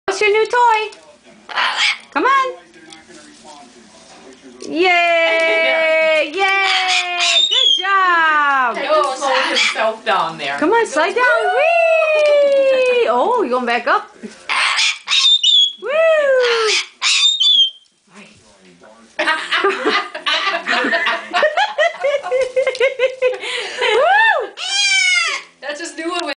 Solo the Bare Eyed Cockatoo playing at my moms house
Here he is just being himself.